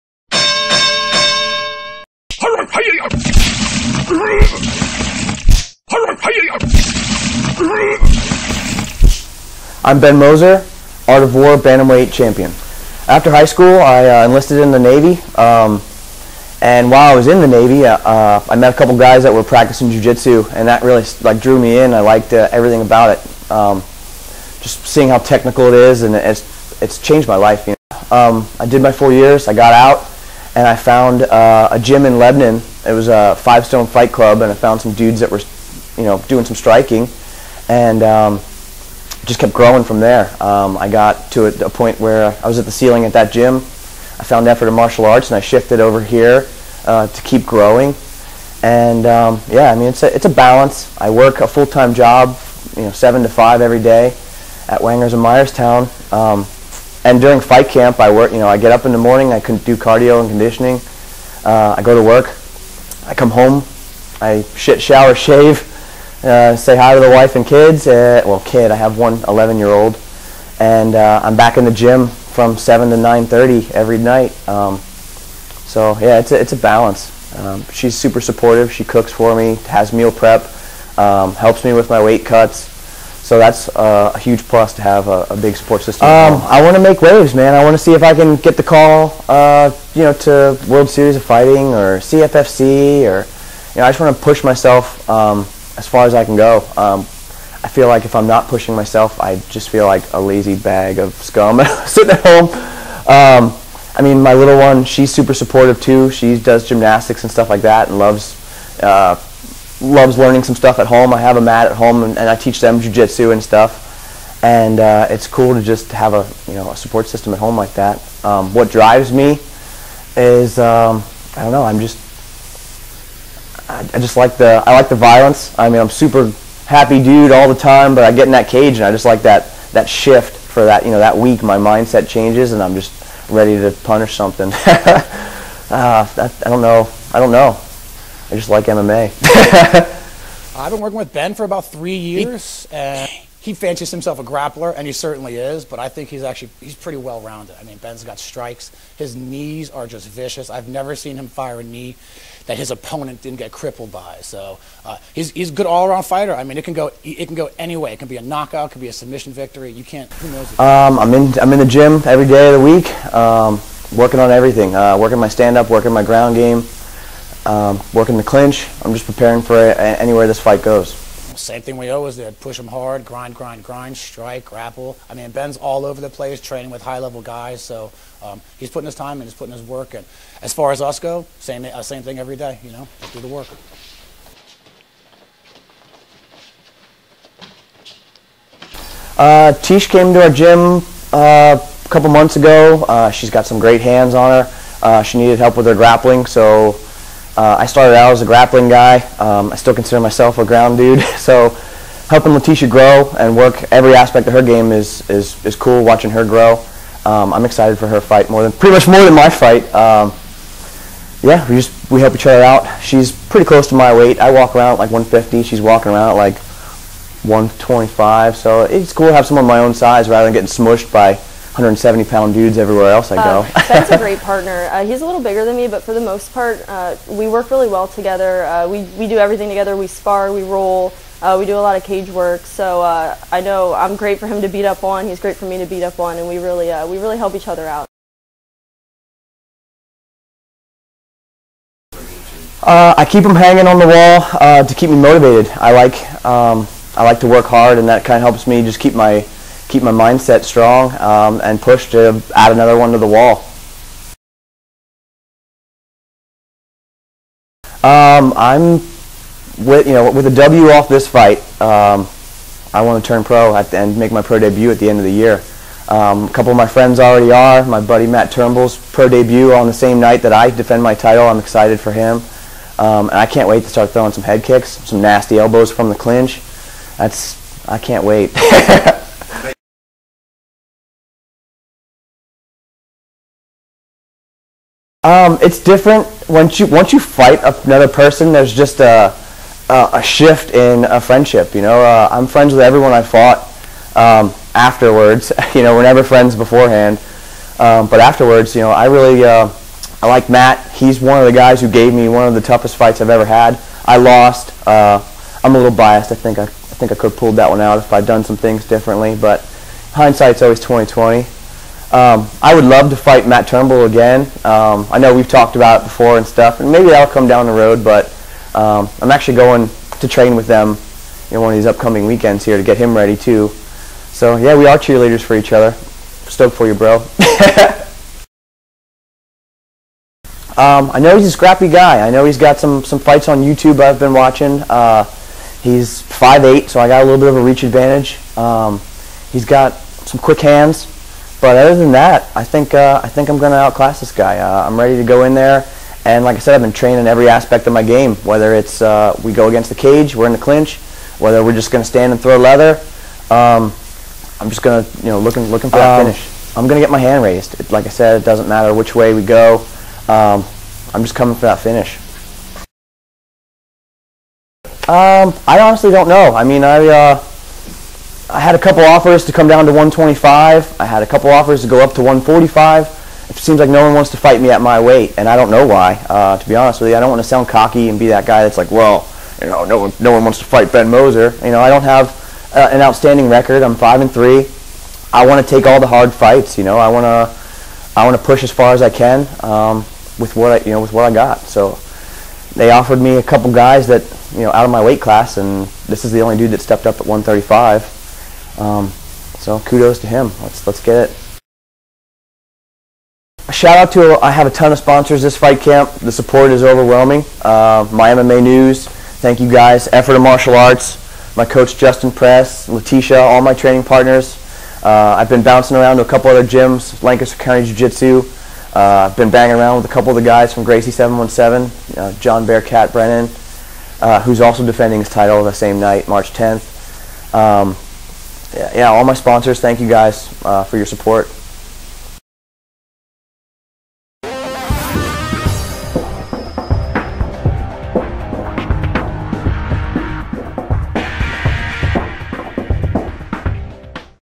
Ben Moser eyes rematch with friend, fellow AOW champion, Matt Turnbull
Ben Moser will look to defend his Art of War Cage Fighting bantamweight title for a second time when he meets Chris Sanchez at AOW6 this Saturday night at the Lanco Fieldhouse in East Petersburg, PA.